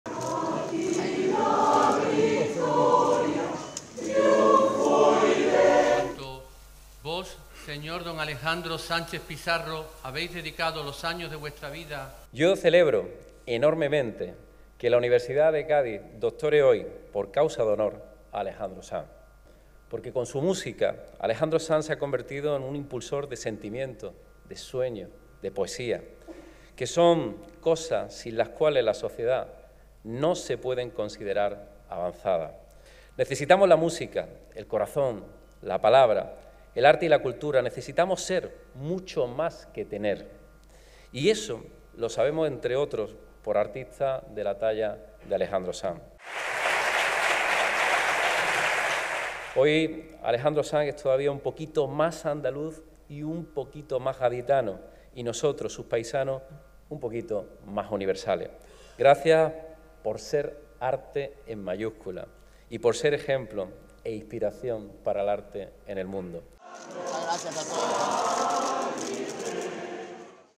Juanma Moreno asiste al acto de investidura de Alejandro Sanz como Doctor Honoris Causa
El presidente de la Junta, Juanma Moreno, ha asistido hoy al acto de investidura de Alejandro Sanz como Doctor Honoris Causa por la Universidad de Cádiz al que ha definido como ejemplo de inspiración para el arte e impulsor de sueños y sentimientos que son esenciales para el avance de la sociedad.
Moreno ha aseverado que en actos como éste se unen el conocimiento, la magia, el pensamiento y la emoción que son contrastes que explican el carácter de la tierra andaluza y ha felicitado a Sanz por este importante reconocimiento porque, según ha dicho, “es una manera de reconocer y proclamar solemnemente, desde la más alta instancia del saber, valores y actitudes como el mérito, el esfuerzo, la virtud, la excelencia o el vínculo sagrado con la tierra de sus padres”.
“Cádiz y Andalucía están por todas partes en la obra y en la persona de Alejandro Sanz. En su habla, en la memoria y en los orígenes de su arte”.
Así, ha recordado el discurso de Alejandro Sanz cuando fue nombrado en 2022 Hijo Predilecto de Andalucía y sus emocionadas palabras a sus padres y a su tierra y la sencillez, la belleza y el cariño con que se hizo notar como un andaluz entre andaluces.
De este modo, ha señalado que el reconocimiento de hoy es nuestra forma de cantarle a Alejandro Sanz todas las cosas que hay que decirle de la única manera que podemos y que sabemos. “Necesitamos la música, el corazón, la palabra, el arte y la cultura. Necesitamos ser, mucho antes que tener y eso lo sabemos, entre otros, por artista como Alejandro Sanz”, ha apuntado.
En este punto, ha manifestado que el mundo universitario es plenamente consciente de esa necesidad social y humana y más en Cádiz donde no existe conocimiento ni lucidez posible sin el alma. “La Universidad de Cádiz se doctora hoy, una vez más, en esa otra forma de sabiduría que está en la calle, en el aire, en las canciones…”.